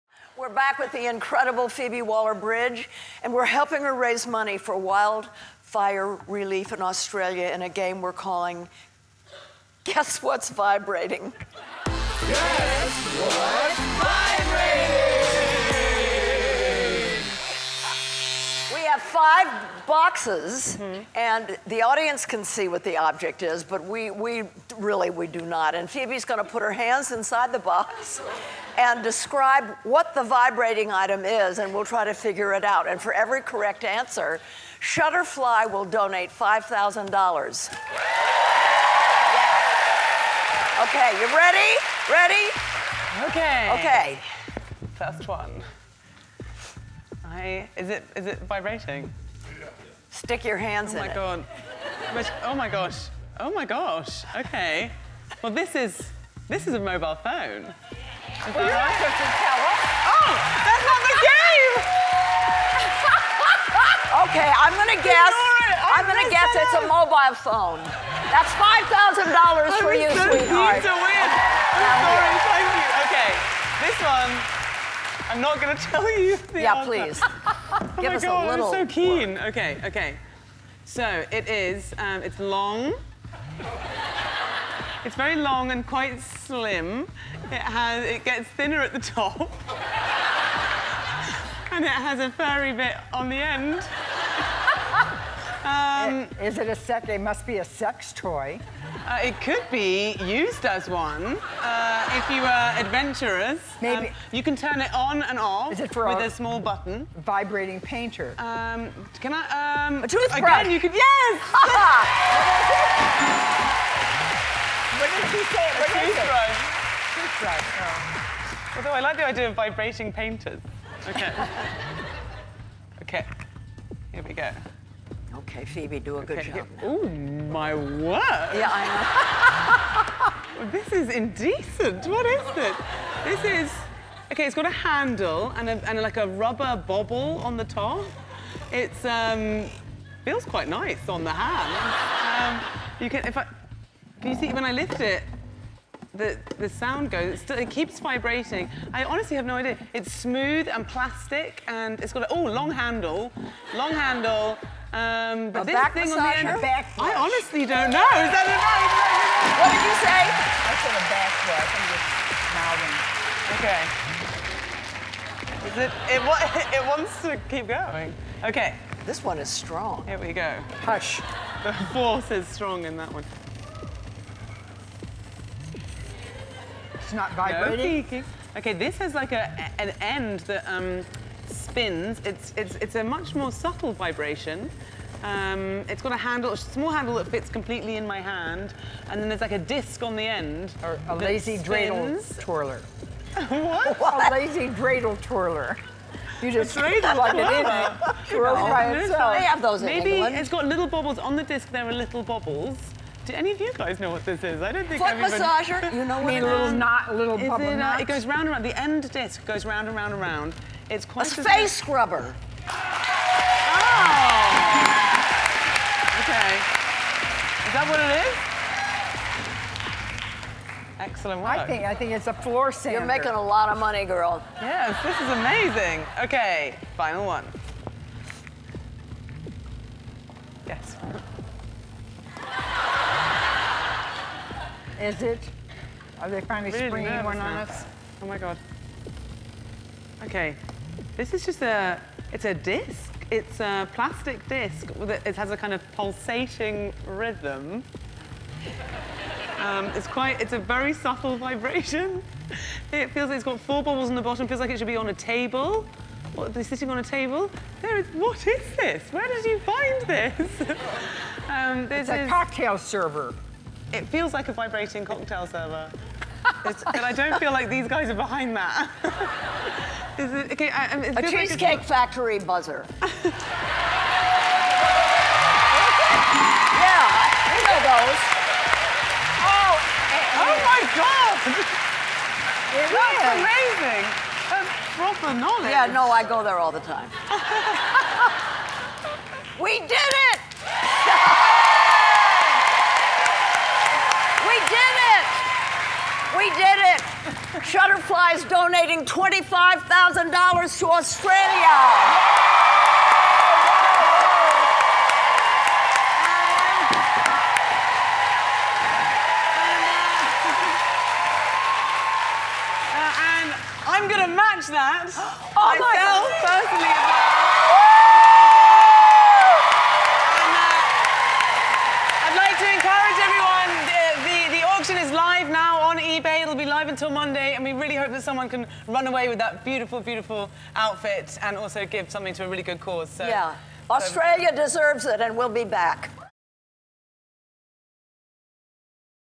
Phoebe Waller-Bridge, Jane Fonda, and Lily Tomlin Play ‘Guess What’s Vibrating’
Guest hosts Jane Fonda and Lily Tomlin played a game called “Guess What’s Vibrating” with Phoebe Waller-Bridge, who had to describe what item was vibrating just by using her hands. After a hilarious round, Phoebe ended up raising $25,000 for Australia Bushfire Relief, thanks to Shutterfly. The actress even matched the amount for a total of $50,000!